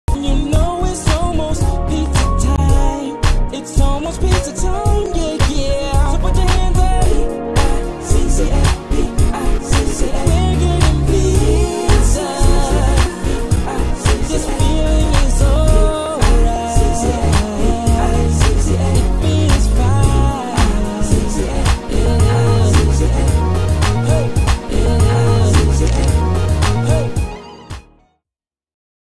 SAY CHEESE! :: THE HUNDREDS
THE HUNDREDS VISITS PIZZANISTA! IN DOWNTOWN LA. THE HUNDREDS IS HUGE.